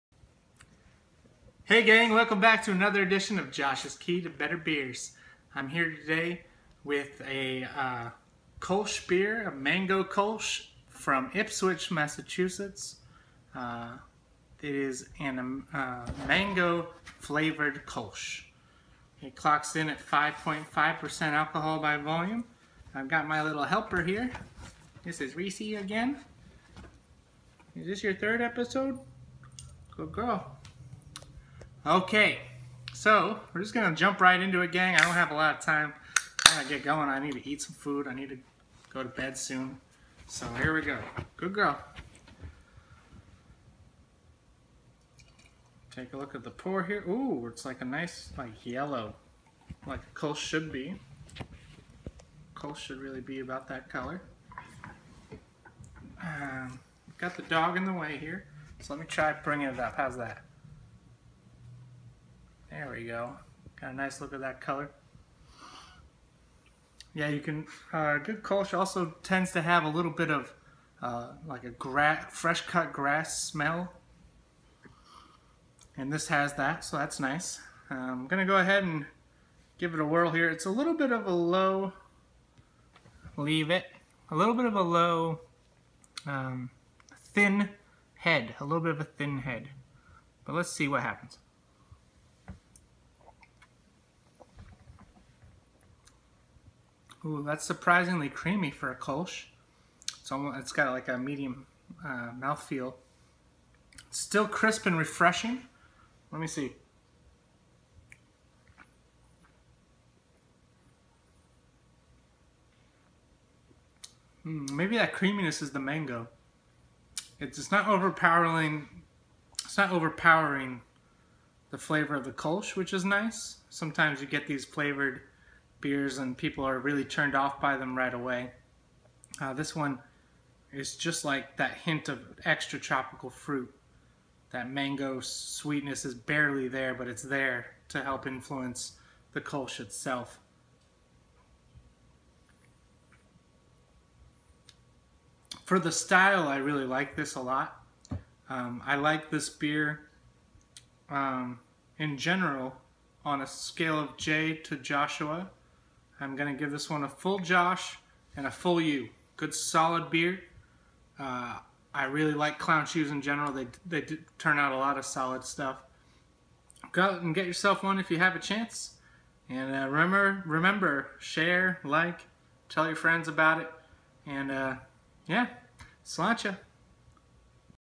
Josh's Key to Better Beers Ep 57: Clown Shoes Mango Kolsch
On today's episode we taste a Mango Kolsch style beer from Massachusetts, one of the elite beer making states!